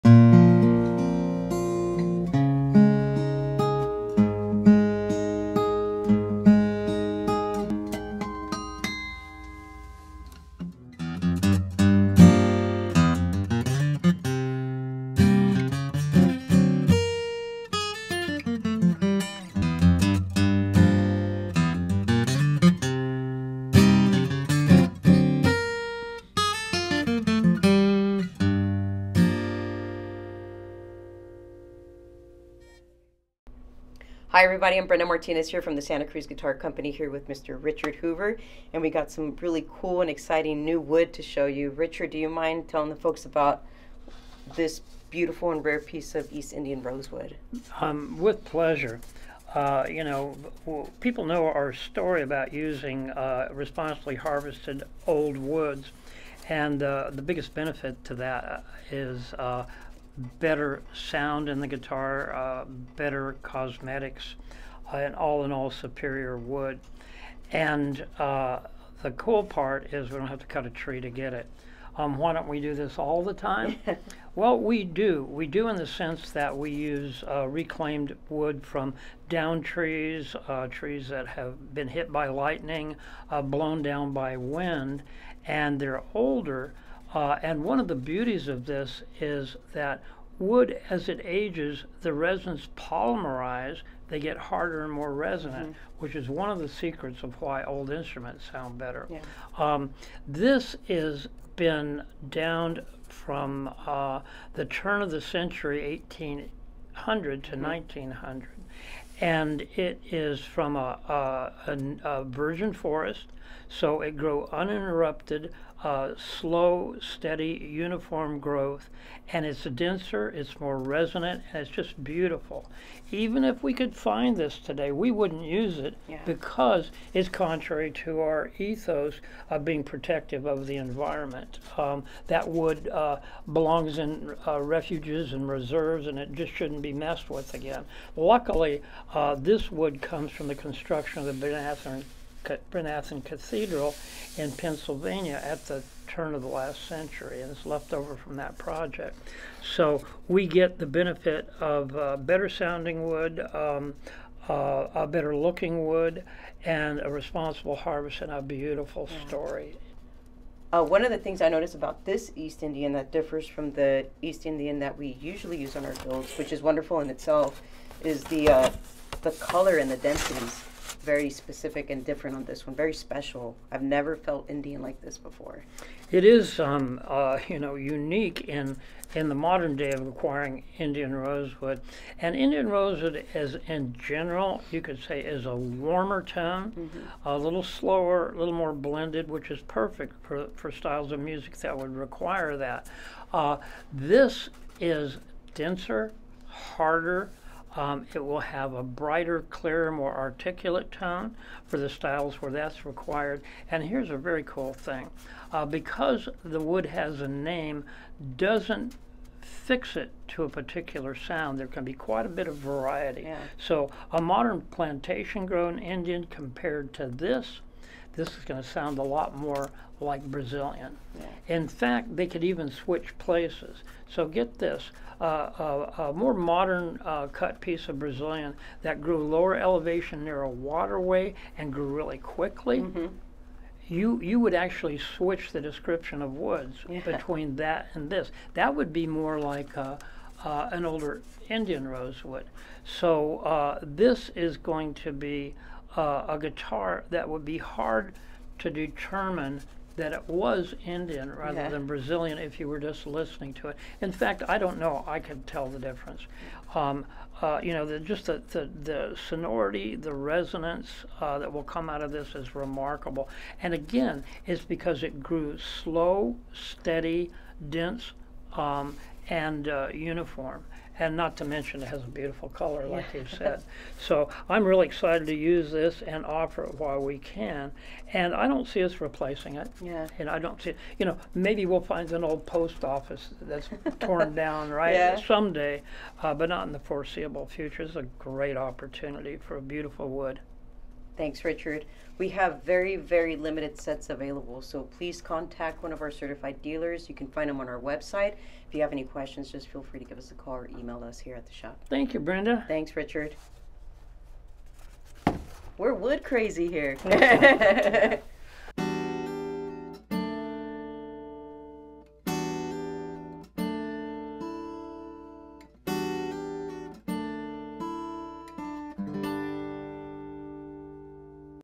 Learn about 100+ y/o EIR from the Bryn Athyn Cathedral (PA) with Richard Hoover and SCGC
Along with the cache of Brazilian Rosewood that saw its debut in our Vault Series D Model at the 2024 Winter Namm in Anaheim, Santa Cruz Guitar Company has acquired East Indian Rosewood that was sourced for the cathedral's construction in the early 1900s (grew and cut in the 1800s) and aged in a stone barn until procurement by Richard Hoover in 2022.

This EIR is unique in its age and slow growth, which will lend a warmer tone similar to Brazilian than a modern day plantation acquired set of EIR. So get this a more modern cut piece of Brazilian that grew lower elevation near a waterway and grew really quickly. You, you would actually switch the description of woods between that and this, that would be more like an older Indian rosewood. So, this is going to be a good hard, that would be hard to determine that it was Indian rather than Brazilian if you were just listening to it.